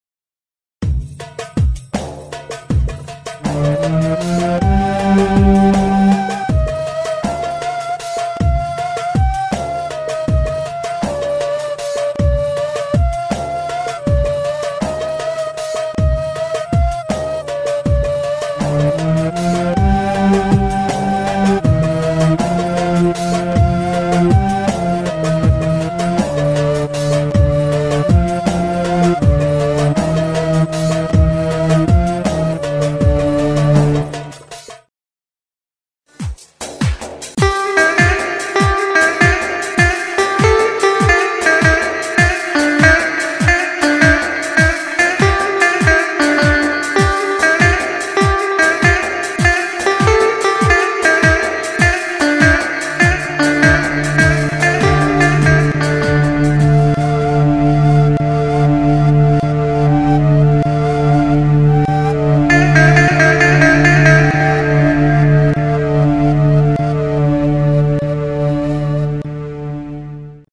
R-org çalma programı yeni ritim güncelleme
5/8
6/8
9/8
ritimler eklendi.